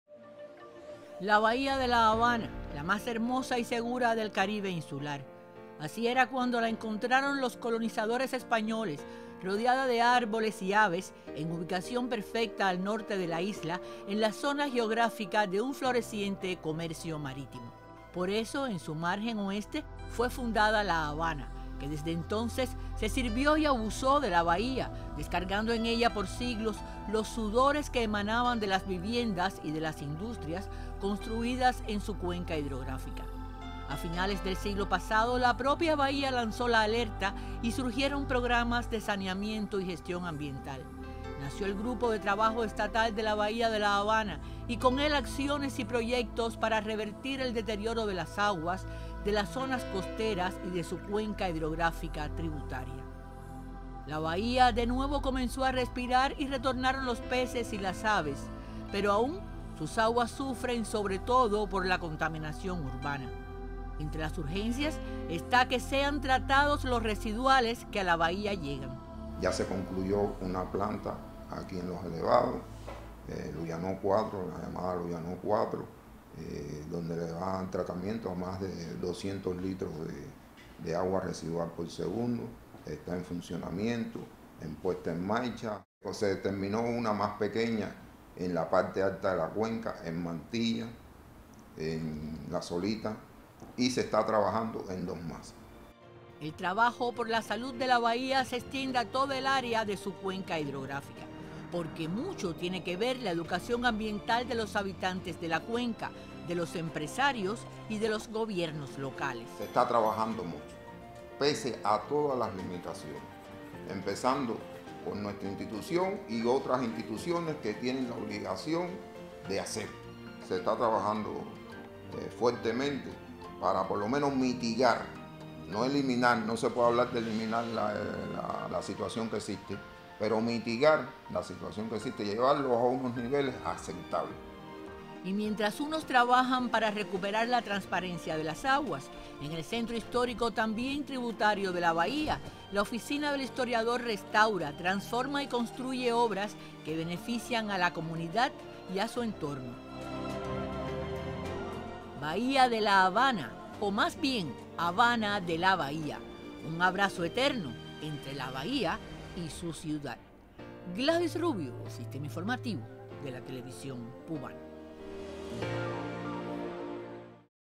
El abrazo eterno de la Bahía y su ciudad
A la fundación y desarrollo de la Villa de San Cristóbal de LaHabana, está estrechamente vinculada su bahía. A pesar de las limitaciones económicas actuales el Grupo de Trabajo Estatal de la Bahía de La Habana, implementa proyectos y acciones que favorecen este ecosistema, amado por quienes viven en la capital.